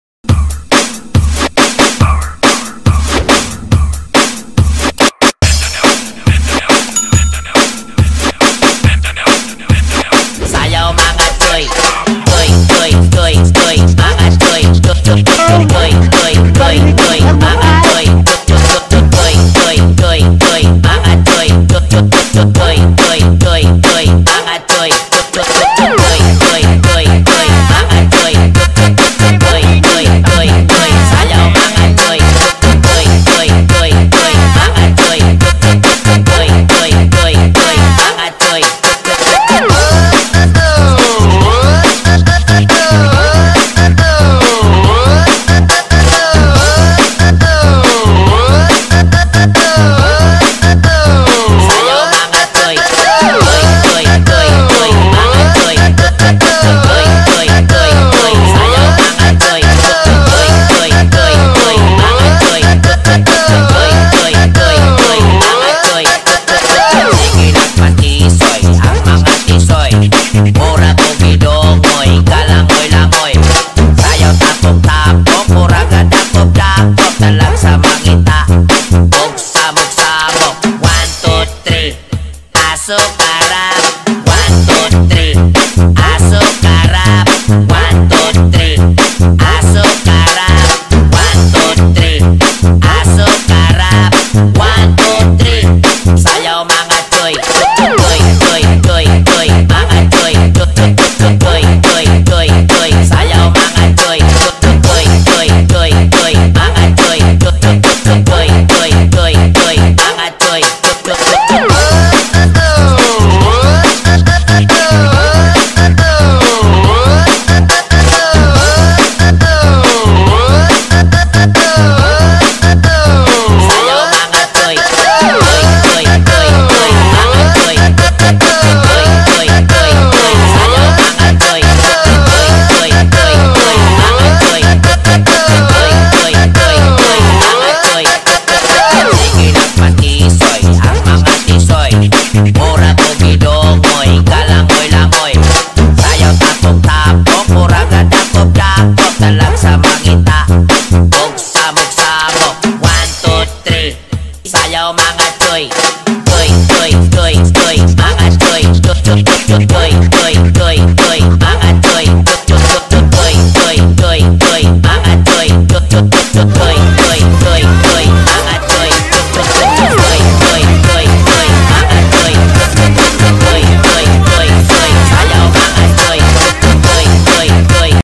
LDR my DAUGHTER DANCING/VIDEOCALL/MESSENGER
pampawala ng stress this is what she have done to us went on top of the chair to made me smile.🤣Iloveuanak ko and i miss u soo much.mwwuuaah